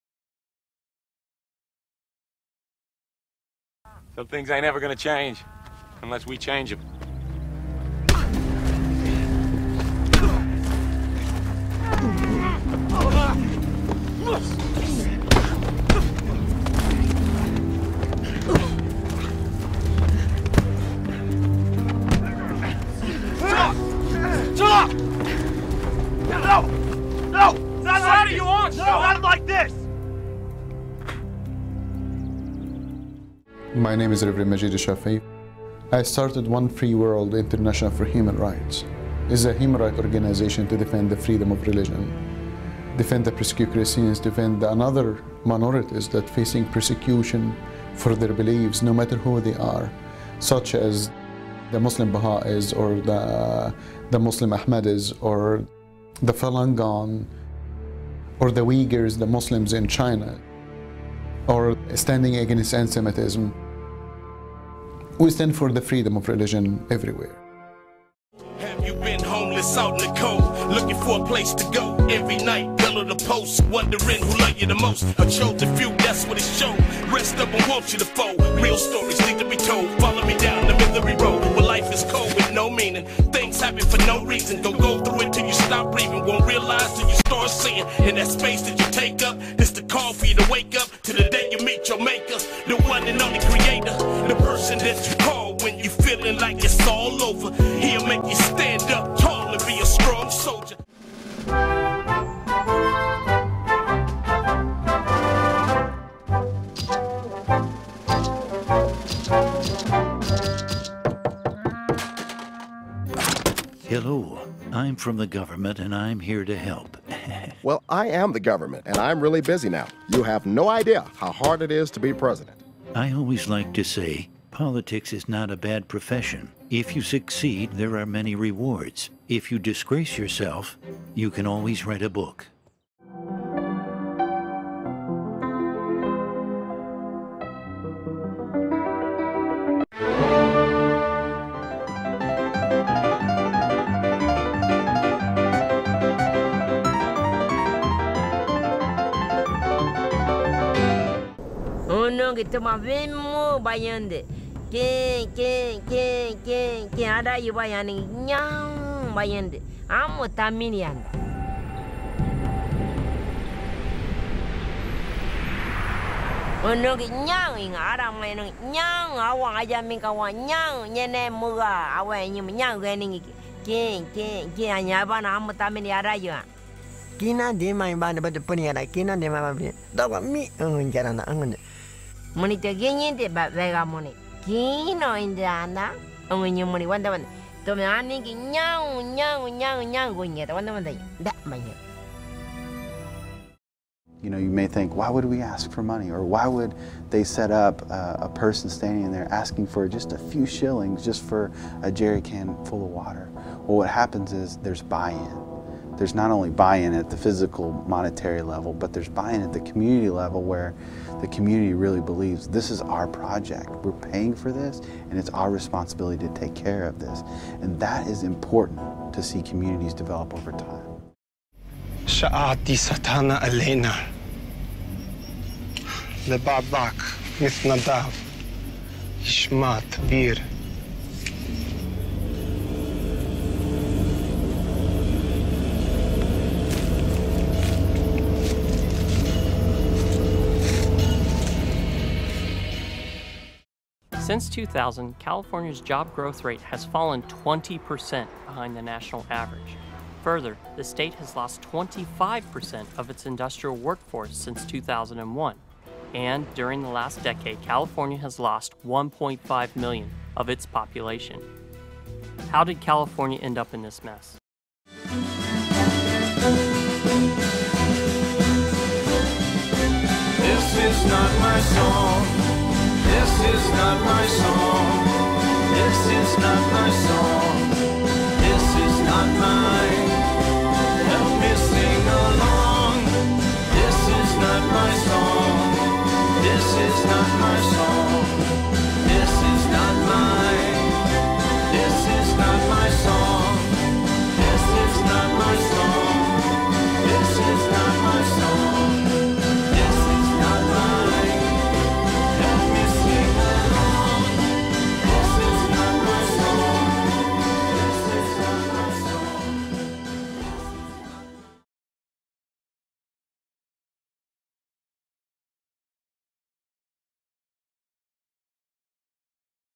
San Diego Christian Film Festival 2011 Sampler 1
2nd Annual San Diego Christian Film Festival, Dec 27-30 at the La Jolla Marriott. This year's Festival includes music videos, shorts, full-length dramas, comedy, & documentaries, a special "Heroes" Night, multiple workshops with award-winning Hollywood insiders and much more.

Select movie samples & trailers
List of nominees
Festival Program
Workshop Speakers and Special Guests
Workshop Schedule